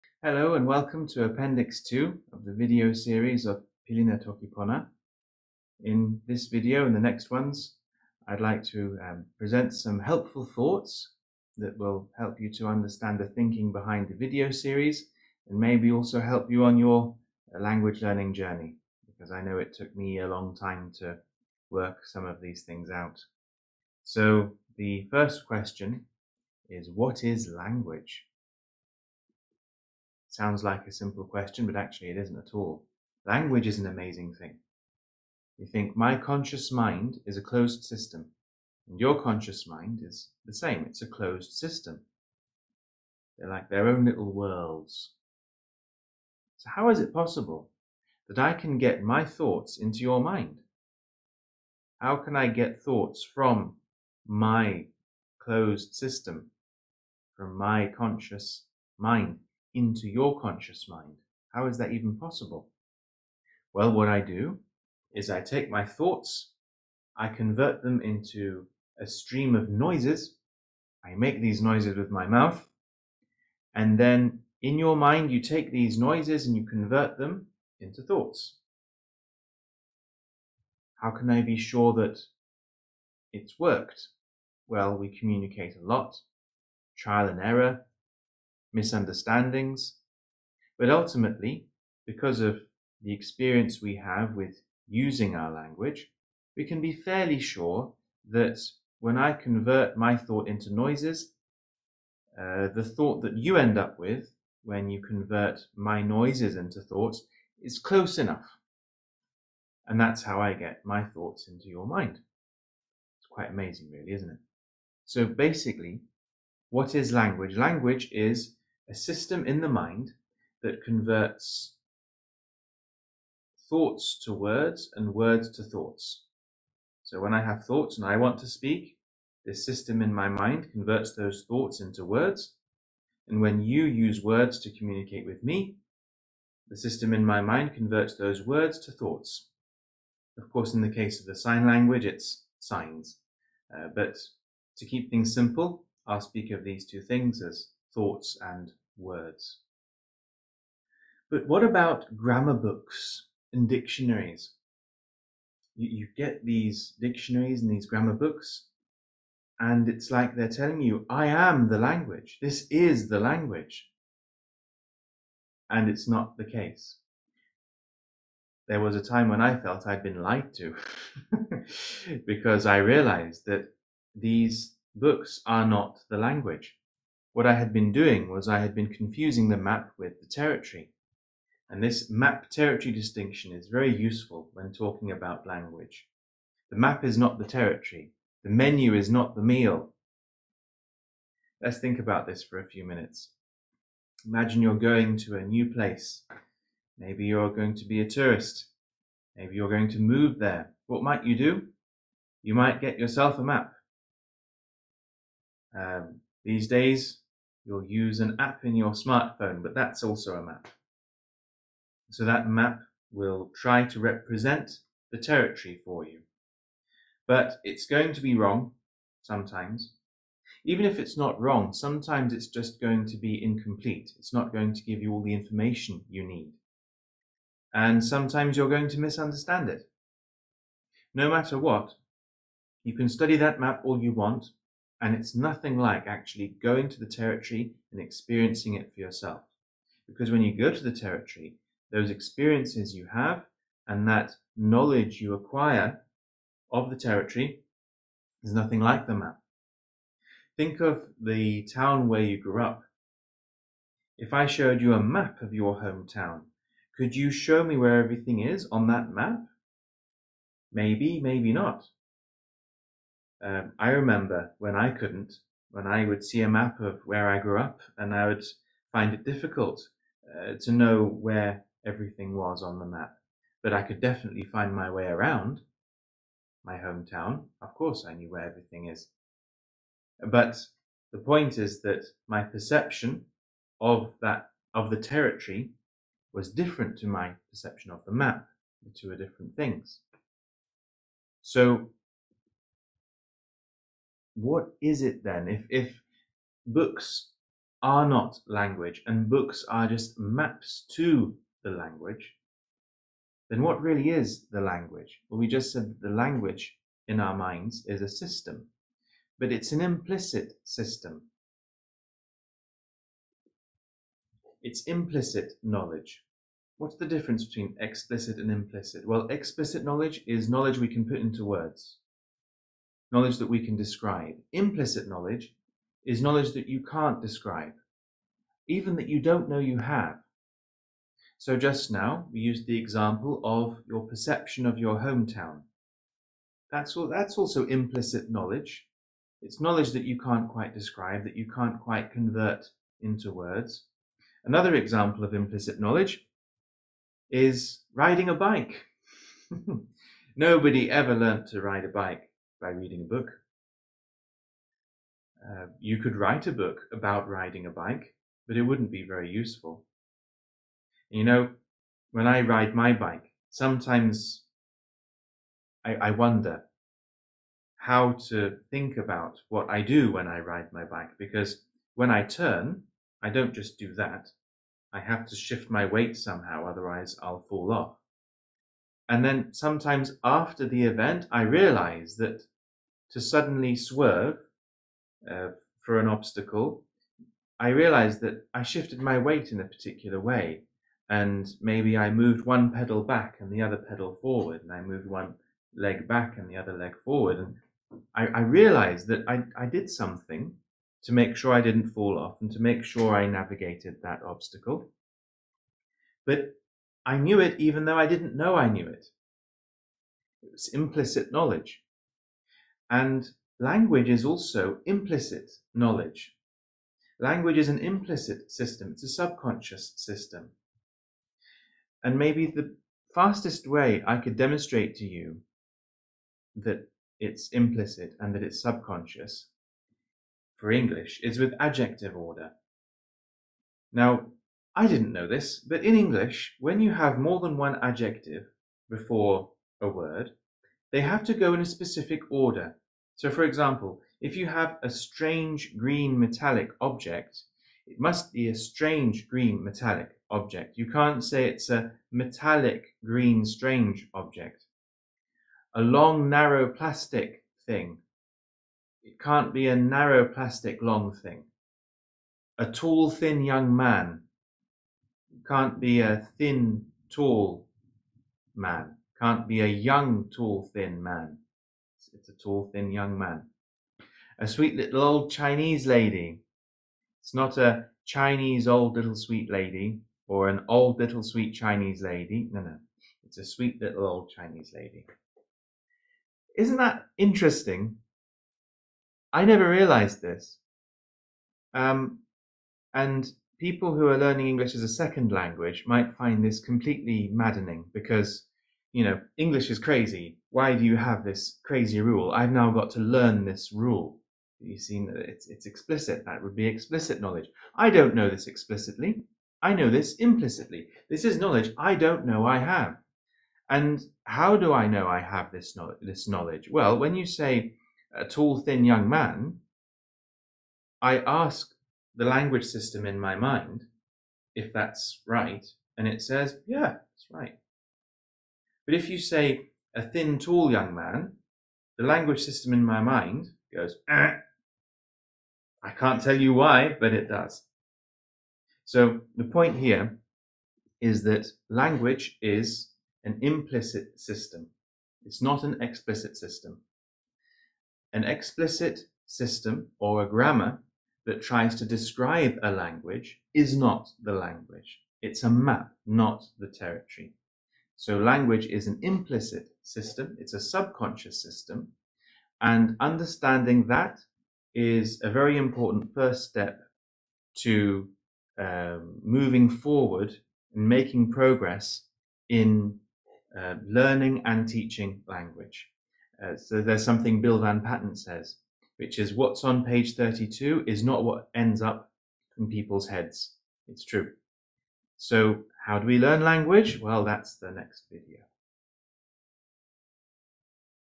A2 What is language? - o pilin e toki pona
Welcome to the video series that tricks your brain into learning a language! Inspired by Automatic Language Growth (ALG), a Story-Listening method adapted for beginners enables effortless acquisition of Toki Pona through comprehensible input. This is a unique opportunity for both language learners and language teachers to experience this process first-hand.